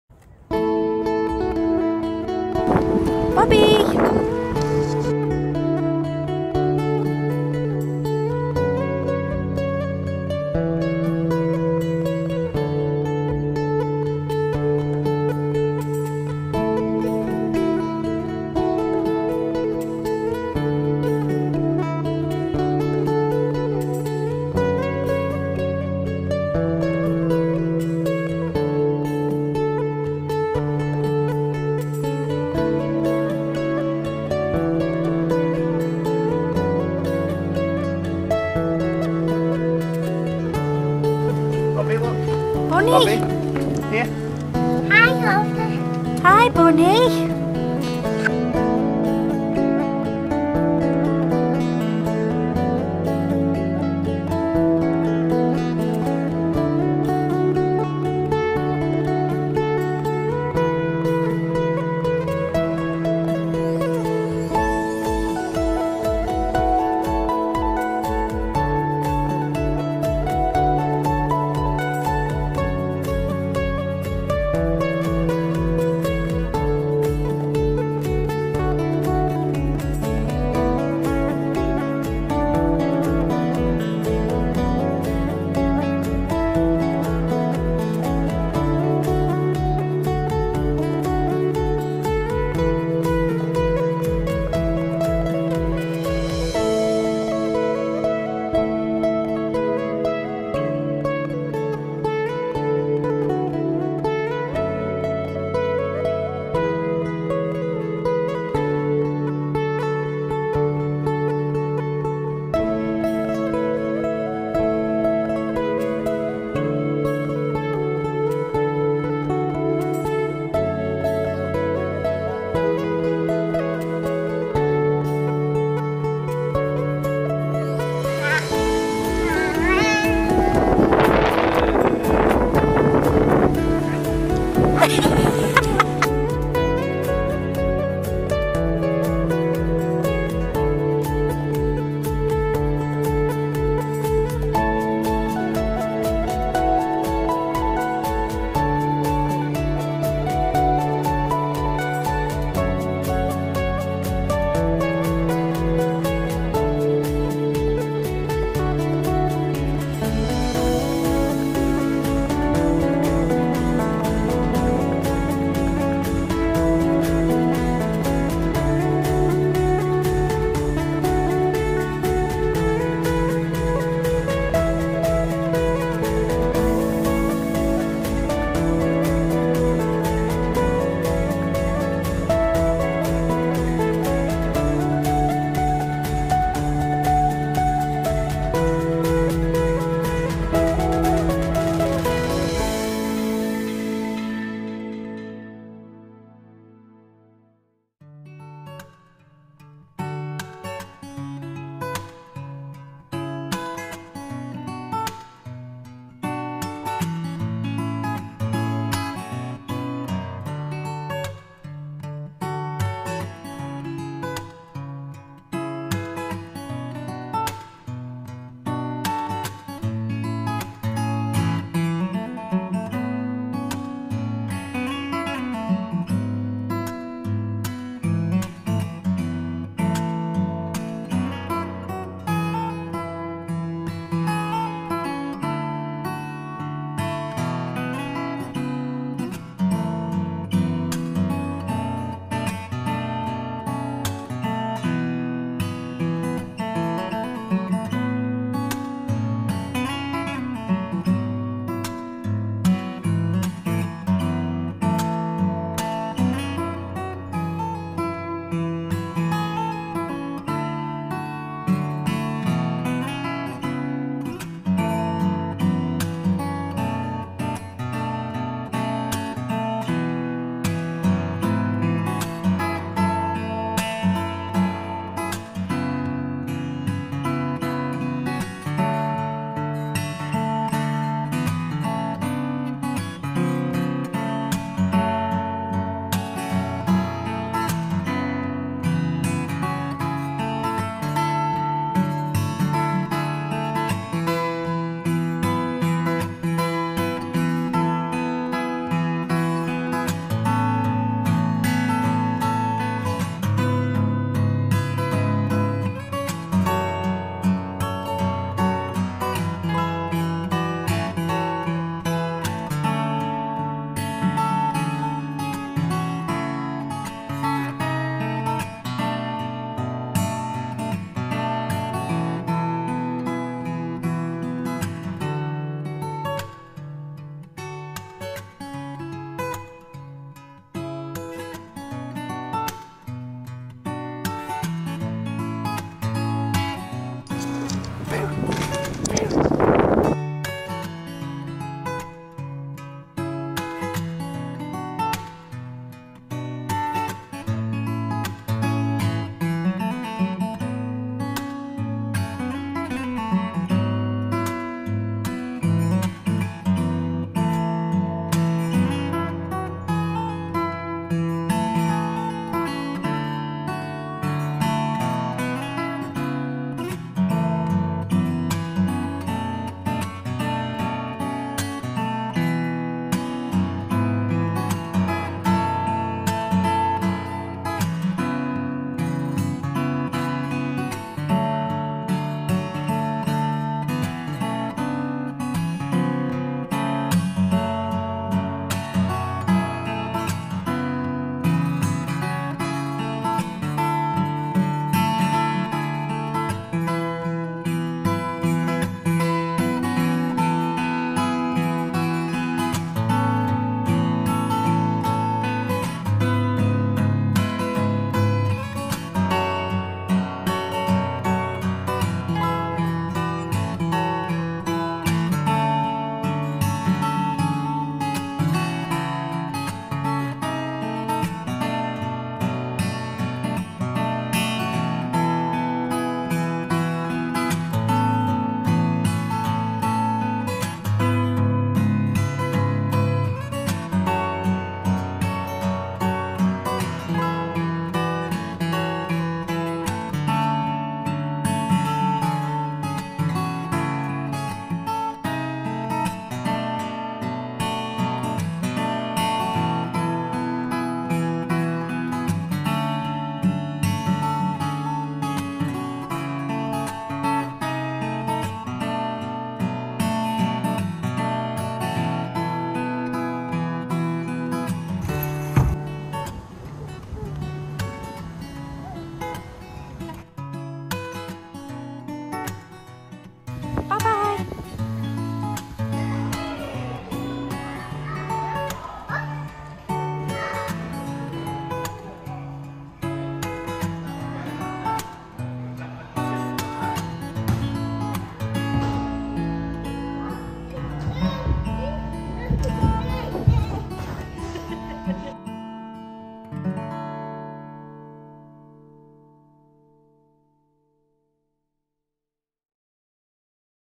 Pumpkin picking & exploring Wheelock Hall Farm 2023
Join myself, Stu & Bobby as we go pumpkin picking and explore, Wheelock Hall Farm in sandbach, Cheshire.
This place is kids heaven!!
Pumpkin patch, cafe, gift shop, farm shop, sooooo many outdoor play areas, farm animals, indoor play and so much more!
If you have any questions please pop them in the comments and of course I will answer them 🥰.

📍Sandbach, Cheshire